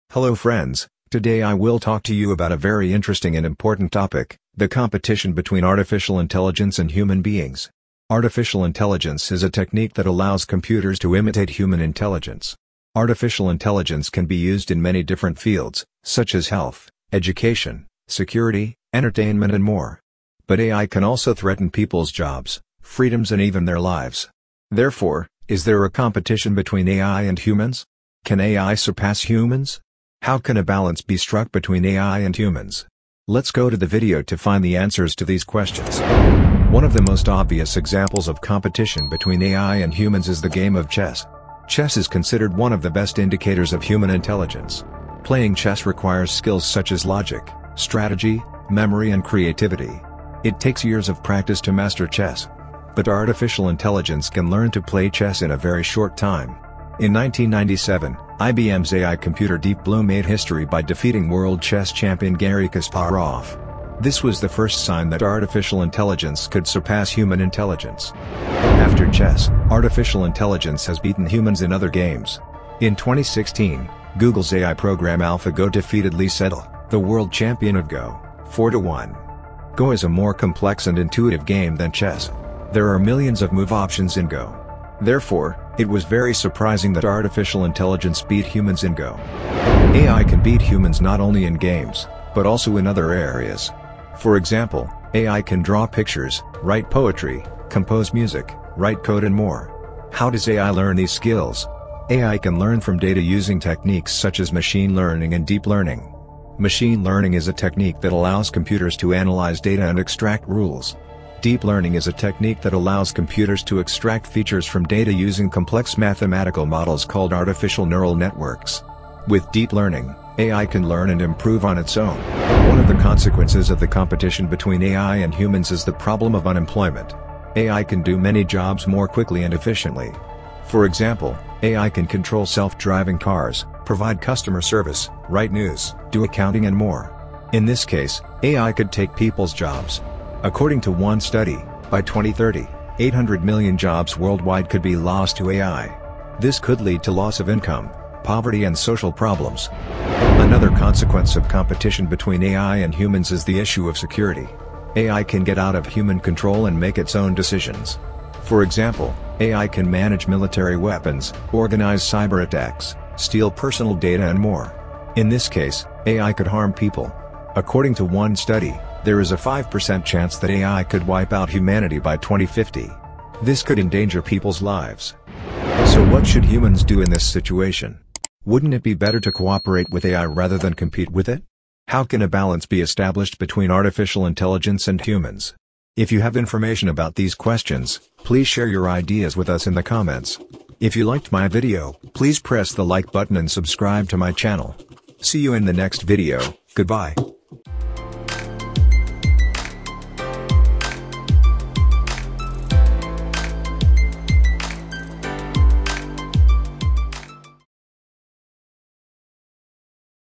Is ARTİFİCİAL İNTELLİGENCE superior to HUMANS? / What is Ai?
Hello friends, today I will talk to you about a very interesting and important topic: The competition between artificial intelligence and human beings. This is a very controversial and intriguing topic in terms of both science and philosophy. In this video, we will look for answers to questions such as what is artificial intelligence, how does it work, in which areas is it used, can it surpass human intelligence, can it harm humans, can it cooperate with humans. We will also discuss what needs to be done to strike a balance between AI and humans.

Artificial intelligence is a technique that allows computers to imitate human intelligence. AI can learn from and improve data using techniques such as machine learning and deep learning. AI can be used in many different fields, for example in healthcare, education, security, entertainment and more. But AI can also threaten people's jobs, freedoms and even lives. Therefore, is there a competition between AI and humans? Can AI surpass humans? How can a balance be struck between AI and humans? Let's watch the video to find the answers to these questions!

After watching this video, I hope you will learn more about artificial intelligence.

The music in this video is from pixabay.com.